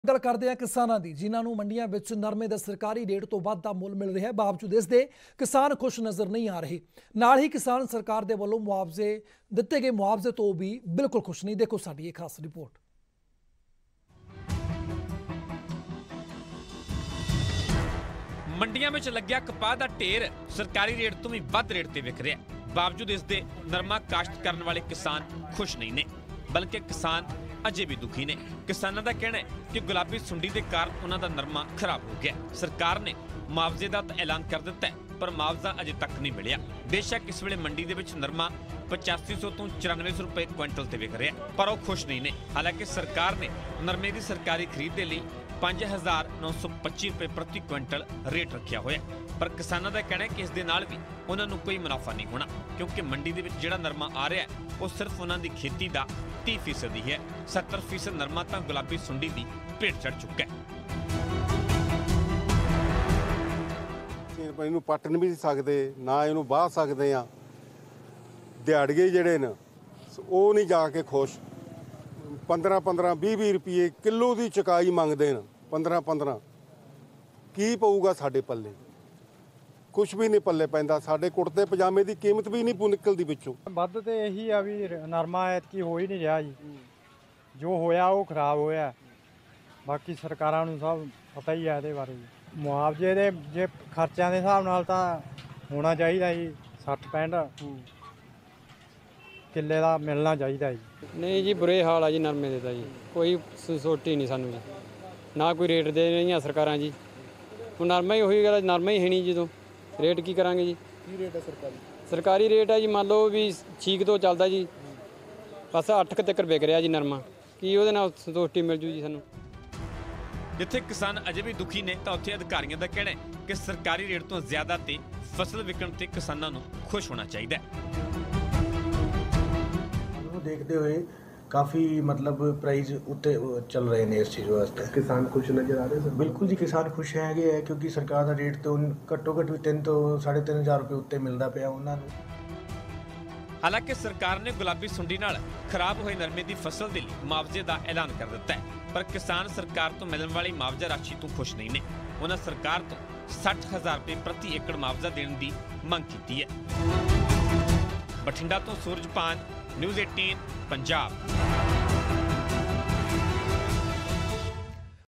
Bathinda News : ਨਰਮੇ ਦੇ ਵੱਧ ਰੇਟ ਮਿਲਣ 'ਤੇ ਵੀ ਕਿਸਾਨ ਕਿਉਂ ਨਹੀਂ ਖੁਸ਼ ? | News18 Punjab
ਕਿਸਾਨਾਂ ਨੂੰ ਮੰਡੀਆਂ ਵਿੱਚ ਨਰਮੇ ਦਾ ਸਰਕਾਰੀ ਰੇਟ ਤੋਂ ਵੱਧ ਦਾ ਮੁੱਲ ਮਿਲ ਰਿਹਾ ਹੈ। ਬਾਵਜੂਦ ਇਸਦੇ ਕਿਸਾਨ ਖੁਸ਼ ਨਜ਼ਰ ਨਹੀਂ ਆ ਰਹੇ। ਨਾ ਹੀ ਕਿਸਾਨ ਸਰਕਾਰ ਵੱਲੋਂ ਐਲਾਨੇ ਮੁਆਵਜ਼ੇ ਤੋਂ ਖੁਸ਼ ਹਨ। ਵੇਖੋ ਰਿਪਰੋਟ।